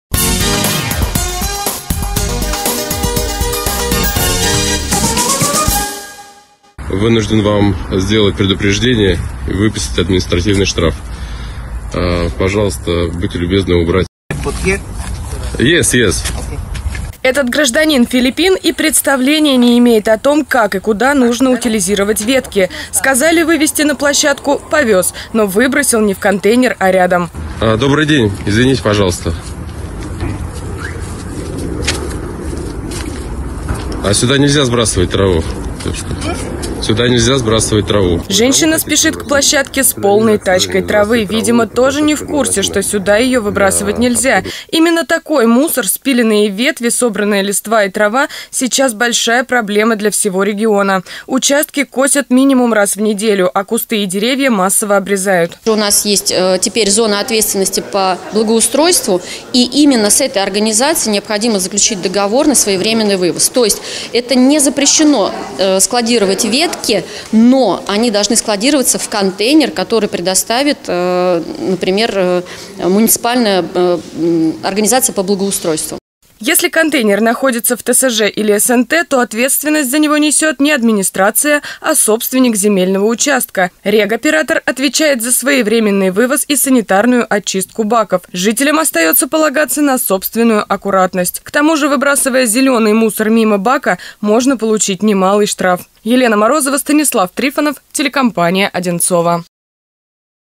Особенности утилизации
В округе продолжаются рейды по борьбе с нарушителями правил утилизации крупногабаритных отходов. Спиленные ветви деревьев и скошенная трава – тоже мусор, причем негабаритный.